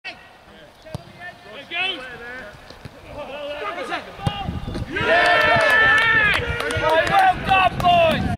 Hindhead 1st Goal
Hindhead open the scoring with a nice finish from a corner